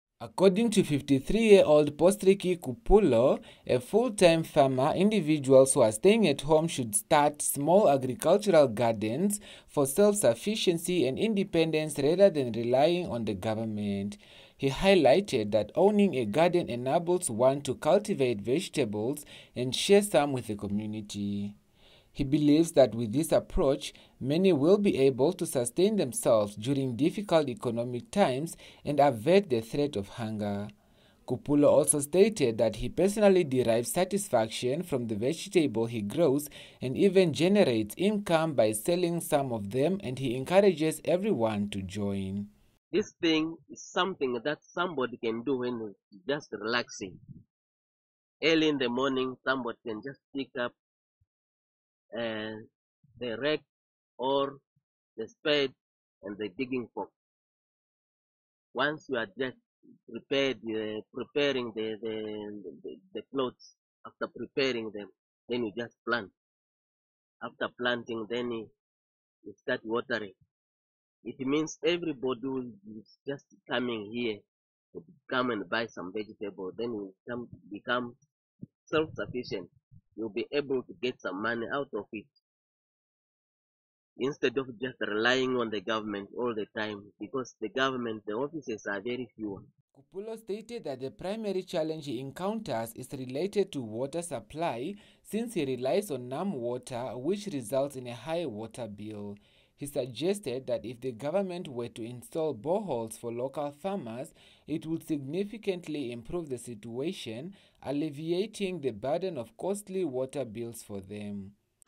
Local farmer urges Namibians to start small gardens for survival
Watch on oneafrica.tv, DStv or GOtv at 19:00 daily
By Jackson Kunatela

A local farmer from Kozo village in Katima Mulilo in the Zambezi region encourages Namibians to engage in farming by starting small gardens, which are easier to manage.

Fifty-three-year-old Postricky Kupulo, who is a full-time farmer, emphasises that this approach can alleviate hunger in the country and reduce the reliance on government support.